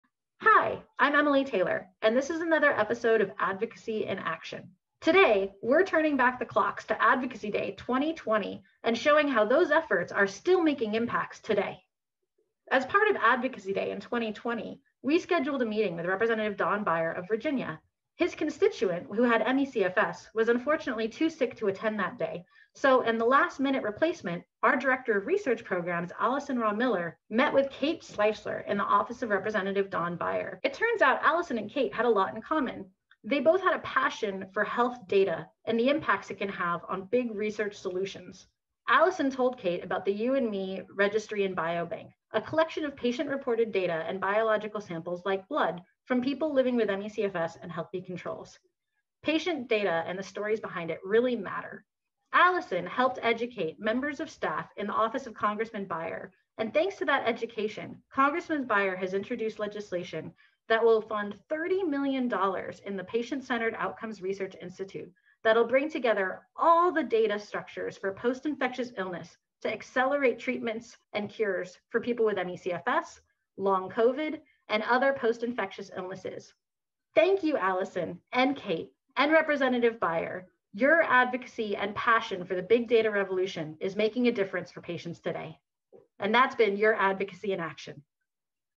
Advocacy in Action: May 12, 2021
At our last Advocacy Day in 2020, Solve M.E. Director of Research Allison Ramiller filled in for an ME/CFS patient too ill to keep their scheduled meeting with a staff member from the office of Rep. Don Beyer (D-VA).

Watch how Allison’s lengthy conversation with the staff member about health data and big research solutions sparked legislative action, just one year later.